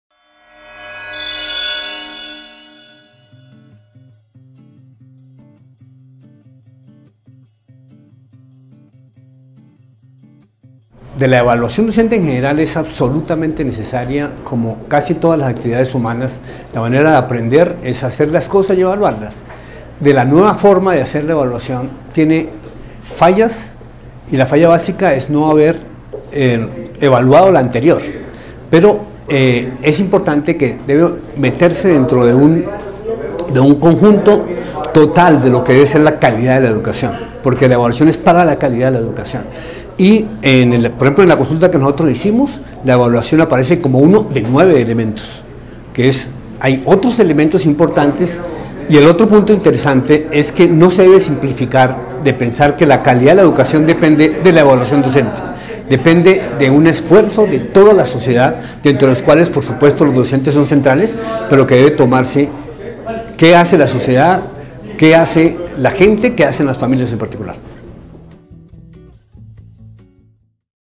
Aspectos relevantes de la nueva evaluación docente
Alfredo Sarmiento Gómez, director de la iniciativa: Misión calidad para la equidad de Naciones Unidas habla sobre los aspectos más relevantes y fallas del nuevo modelo de evaluación docente que se implementa en Colombia.